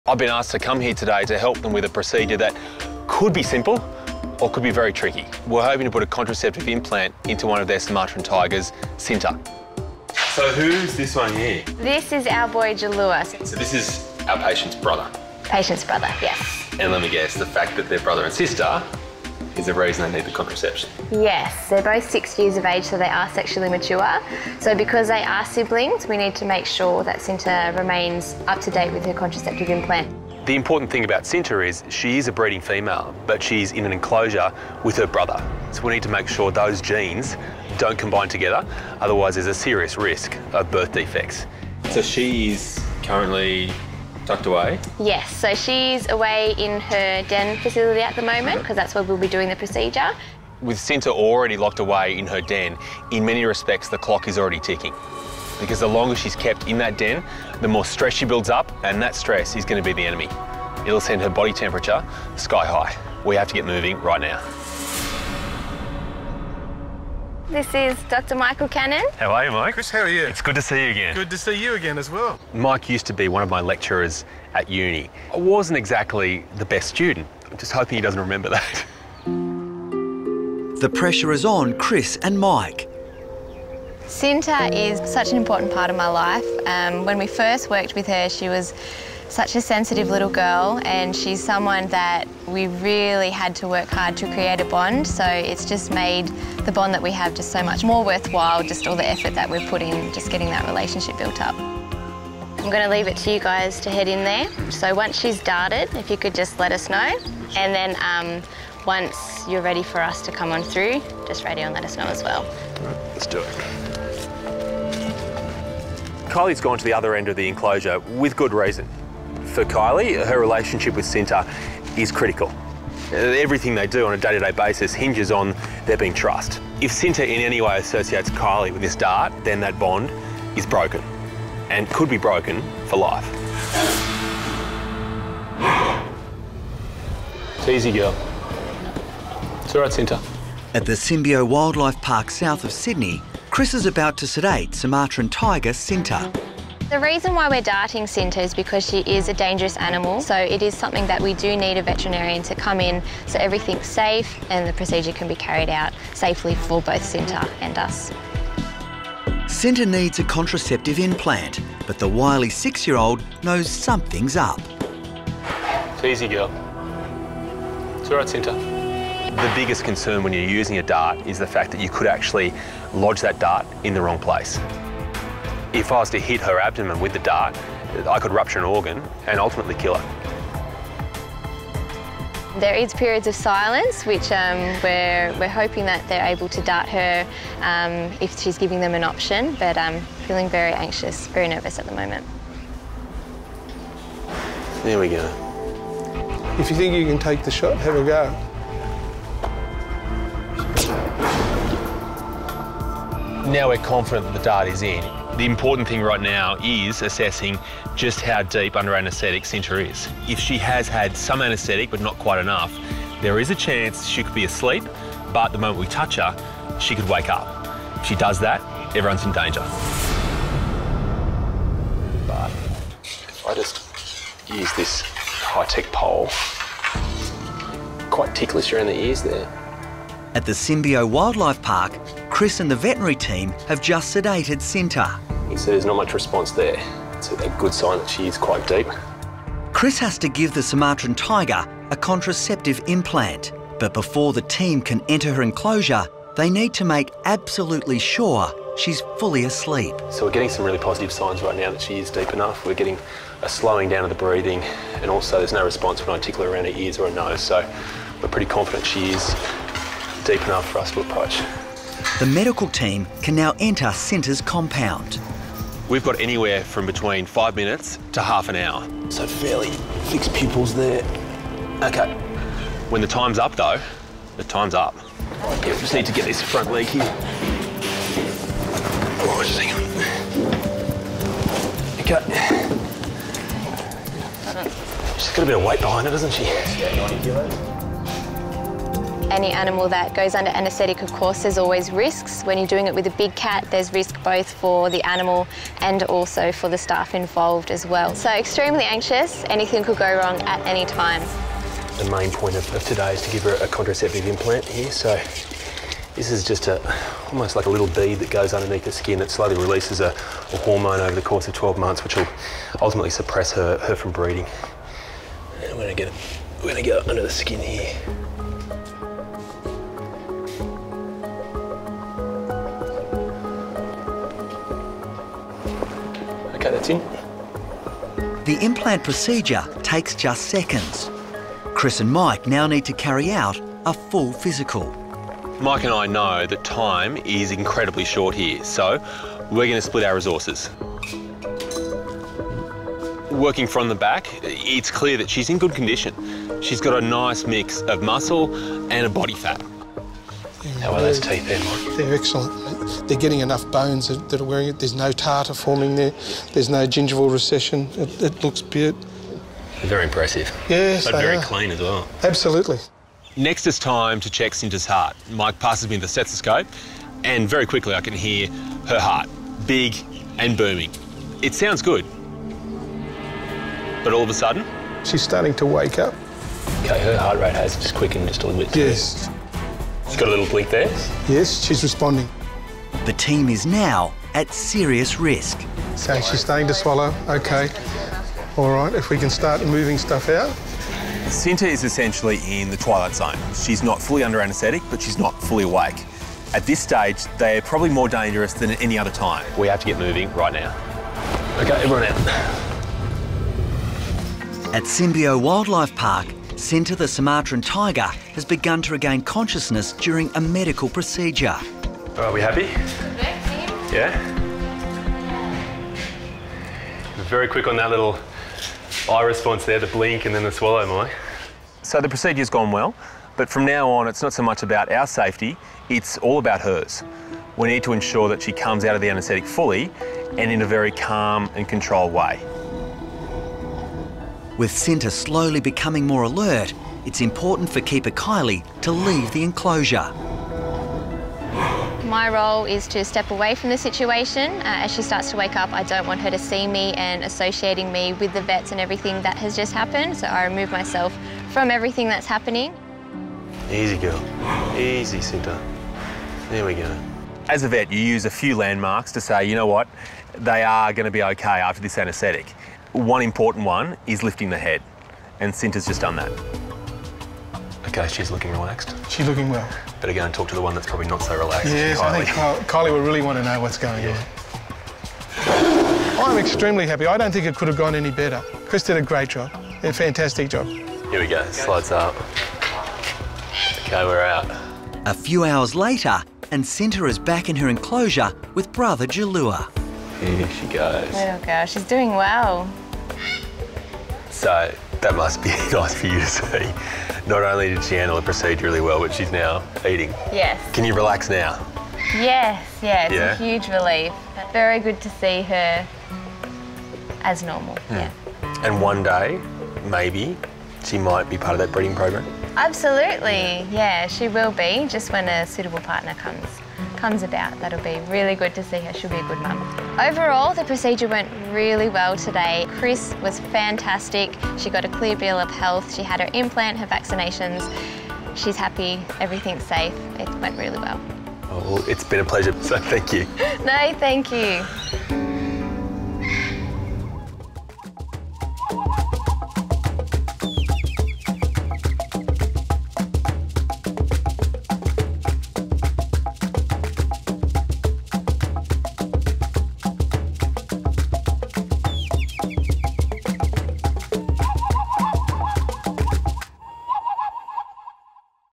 Operating On Giant Sumatran Tiger Before She Wakes Up! 😰 Untamed
Dr Chris needs to enter this Sumatran Tiger enclosure and operate before sedation wears off...